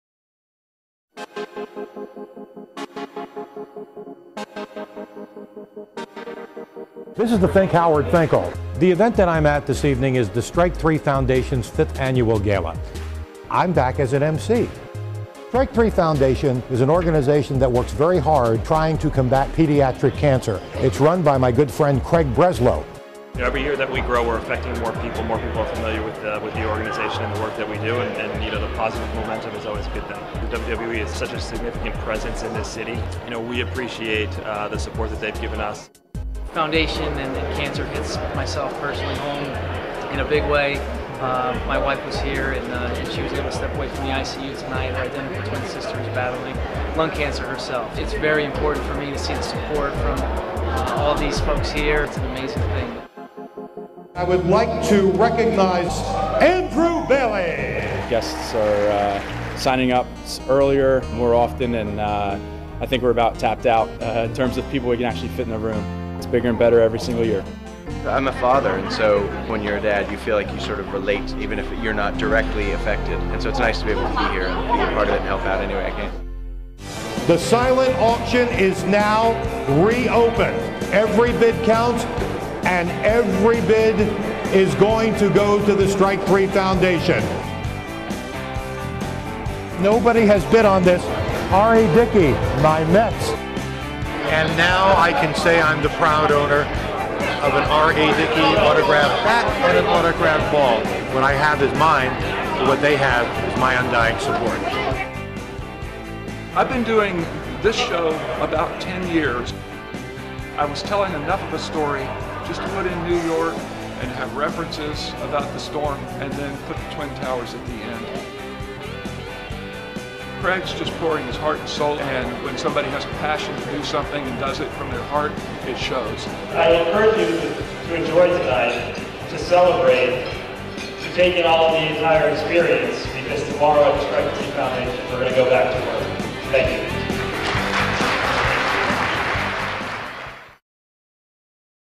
WWE Hall of Famer Howard Finkel and the WWE support the Strike 3 Foundation
The Strike 3 Foundation held their 5th Annual First Pitch Celebrity Gala in Stamford with special host, WWE Hall of Famer, Howard Finkel. Watch to find out more about this amazing event and ever-so-important charity.